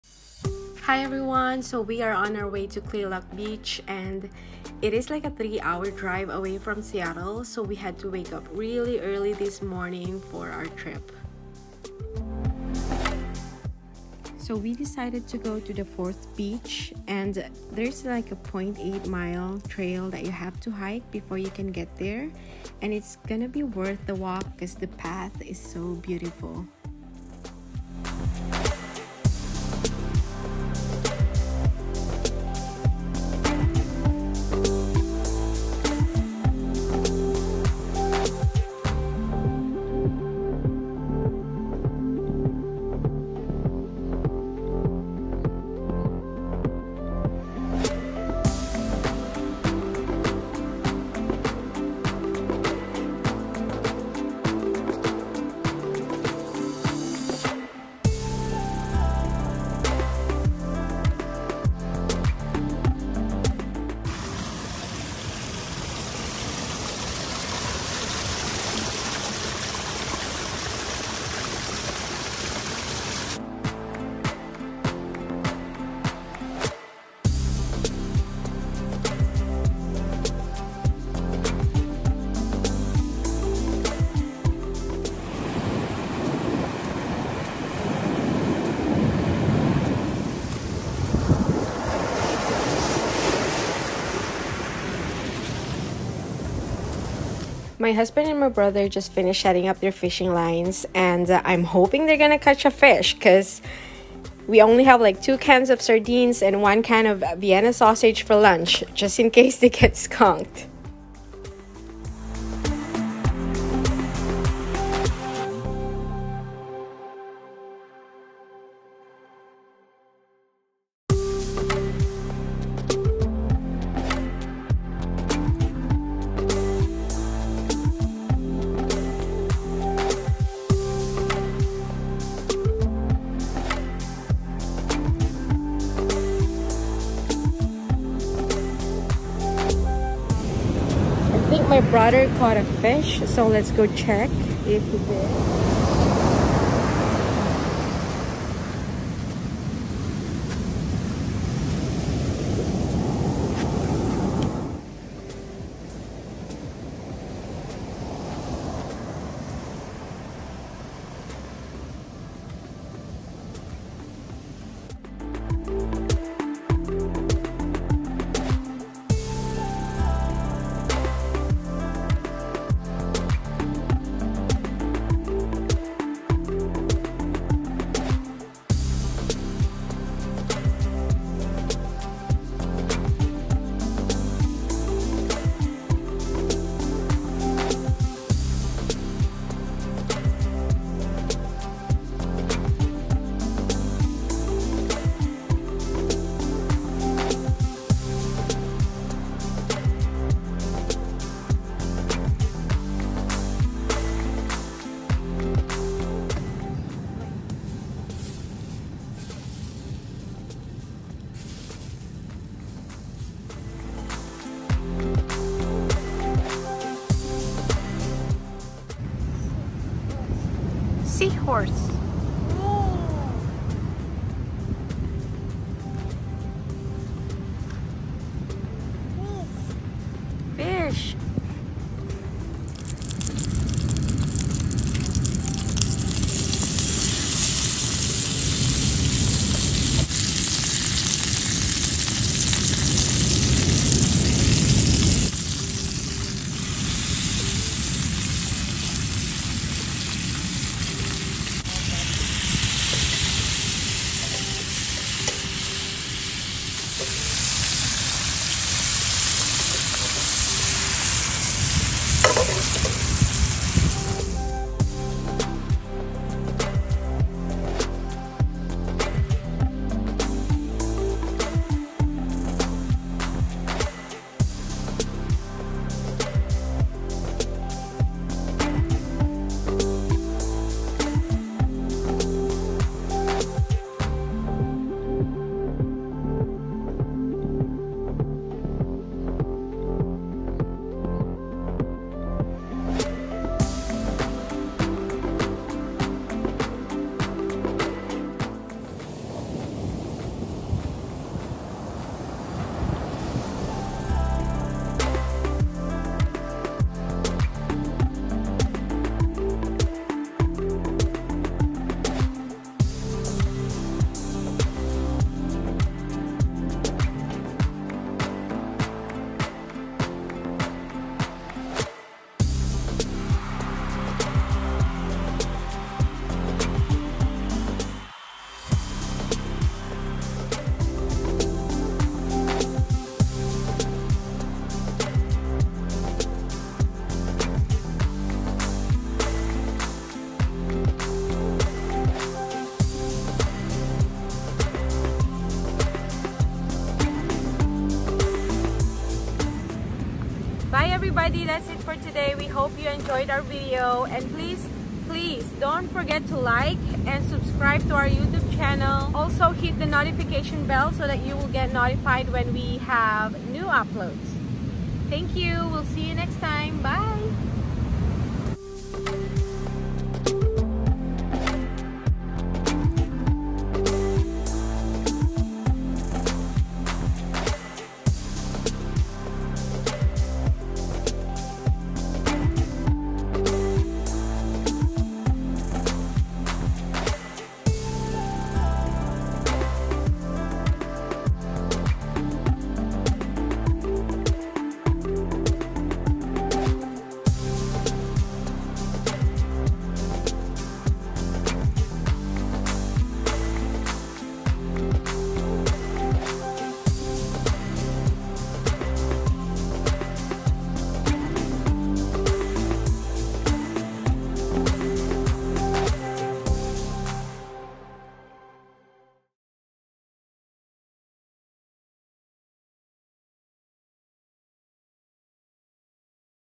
Catch & Cook & Camp: SURF PERCH!!!
A relaxing day, camping & fishing at Kalaloch Beach WA